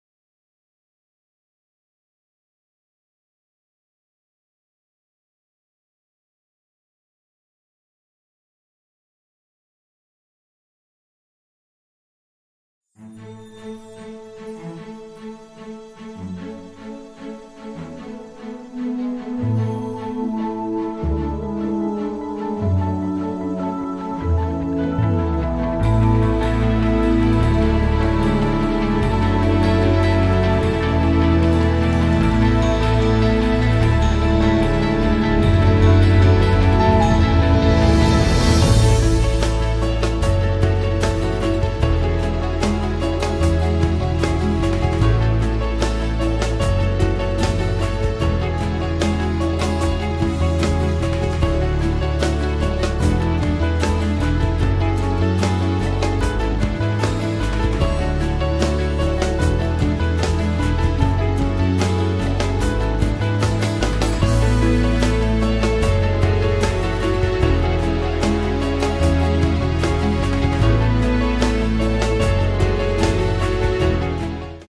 7 MARLA LAKE CITY PLOT FILE FOR SALE
7 Marla plot available at 7 Lakh Down Payment thousand own.
Total Booking Price: 41.90 Lakhs payable in two and half years Installments. 3.49 Lakhs Quarterly.

Location Location Location: Due to start of Lahore Ring Road, widening of Raiwind Road, and very convenient alternate route from Shaukat Khanam Hospital to Raiwind Road adjacent Lake City, the raw land prices for future development in Lake City are rising sharply.

* Located on main Raiwind Road near Adda Plot round about
* Own power generation back up plant (No load shedding)
* Underground wiring/utilities
* Populated area with thousands of families living in
* Masjid, Parks, Cinemax Cinema, Wellness Club and much more.
* Froebel’s International Schools
* All utilities available
* PGA Standard 18-hole golf course at the Lake City Golf Course and Country Club
* Due to Ring Road Development in coming year: 15 minutes approach from Airport & DHA
* Low Risk & High Reward investment
* Recommended for living and both short, long term investment